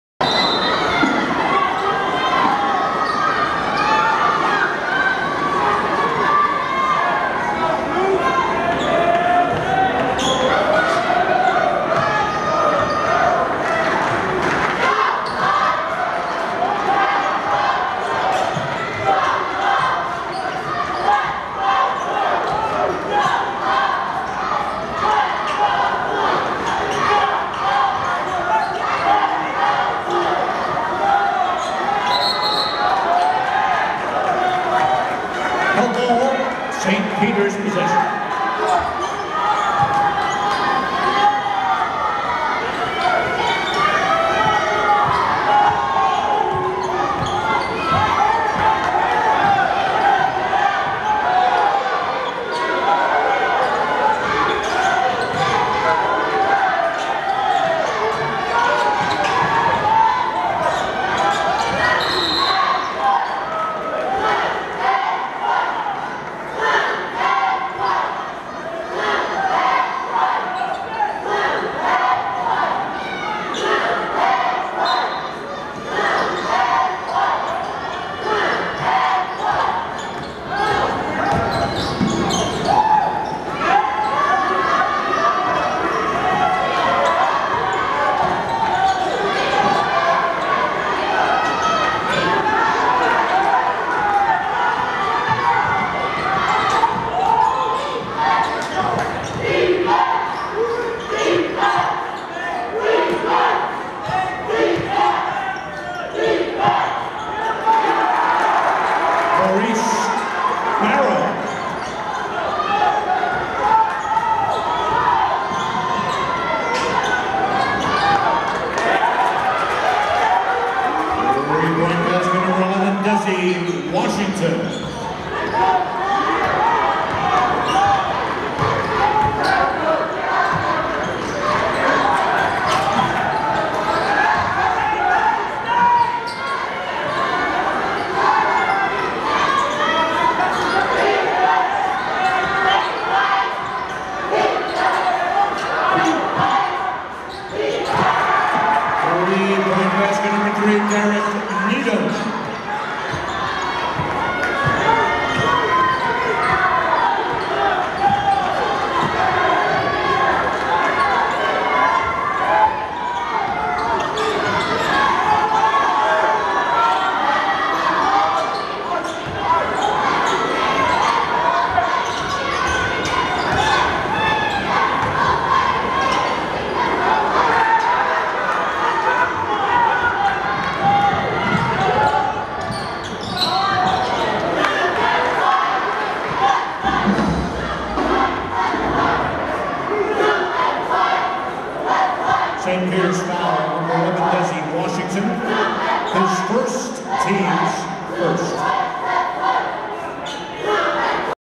Fairfield vs St. Peter's - Men's Basketball - Opening Tip-Off - MAAC Tournament - March 08, 2013
Fairfield Stags vs St. Peter's Peacocks
Opening Tip-Off - Men's Basketball - MAAC Conference Tournament
MassMutual Center  -  Springfield, Massachusetts